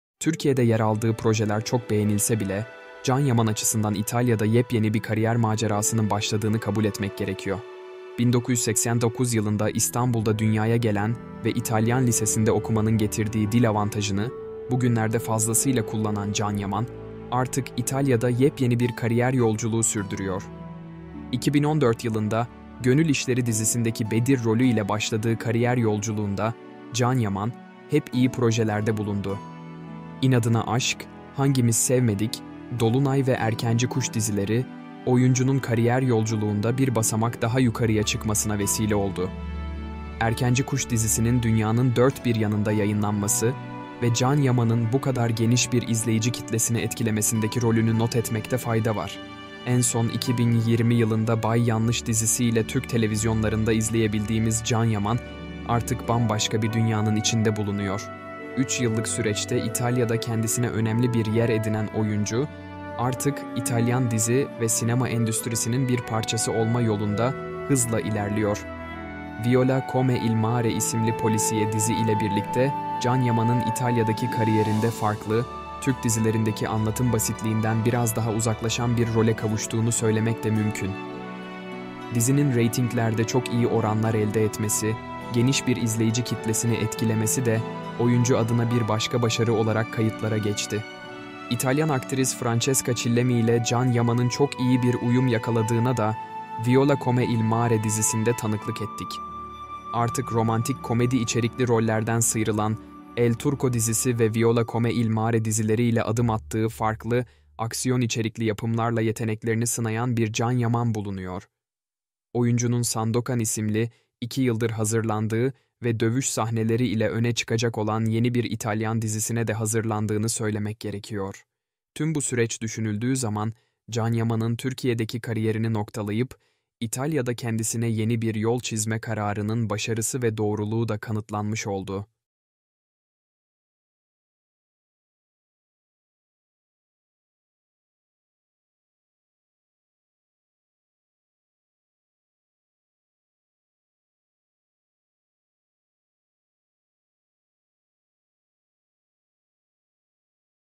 Can Yaman's NEW ITALIAN SERIES
Ünlü oyuncu Can Yaman, uzun süredir İtalya’da yaşamını sürdürüyor. Yer aldığı etkinlikler ile artık magazin gündeminin bir parçası haline gelen Can Yaman, İtalyan kamuoyu tarafından da yakından takip edilen bir star isim haline geldi.

Can Yaman’ın İtalyan kültürü ile uyuşmasının altında, okul yıllarında İtalyan Lisesi’ne giderek hem İtalyanca öğrenmesi hem de bu ülkenin kültürü ile yakın iletişim içine girmesi de önemli bir rol oynamış gözüküyor.

Can Yaman’ın İtalya’da hem televizyon kanallarında hem dergiler hem gazete ve internet sitelerinde yaptığı işlerle sıklıkla yer alması artık normal bir durum haline geldi.
Sandokan isimli dizinin çekimleri nihayet başlarken, Can Yaman hayranları ünlü oyuncunun işlerini yakında takip ediyor. Canale 5’te hem Verissimo hem de Amici programlarında hayranları ile bağının ne kadar güçlü olduğunu bir kez daha ortaya koyan Türk oyuncu, Viola Come il Mare isimli dizi ile yine kendisine hayran bıraktı.
Bu videoda Can Yaman'ın kariyeri ve İtalya'daki dizilerini konu aldık .İyi seyirler..